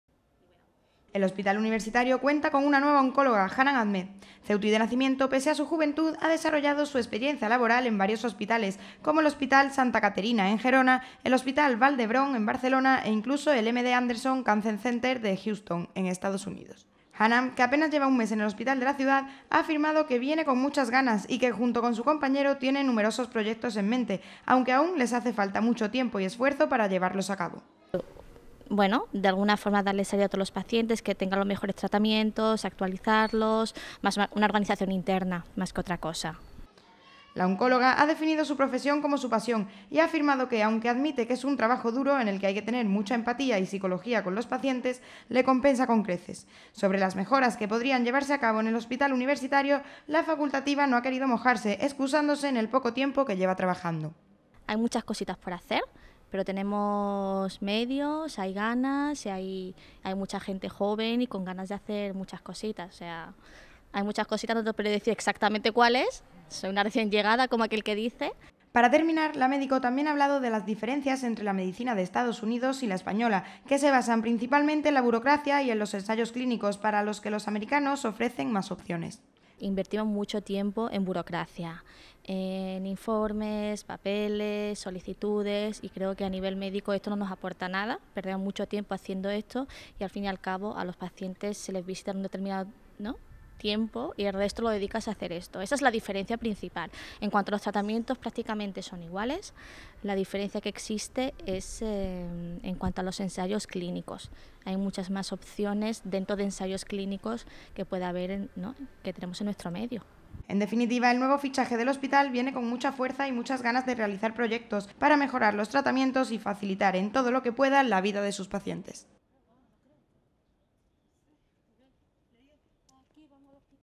Ganas, nuevos proyectos e ilusión, la receta de la nueva oncóloga del Hospital Universitario
Hanan Ahmed, la nueva oncóloga del Hospital Universitario, apenas lleva un mes en su nuevo puesto de trabajo pero asegura que ha llegado con muchas ganas, proyectos e ilusión, para mejorar el área de oncología de la Ciudad.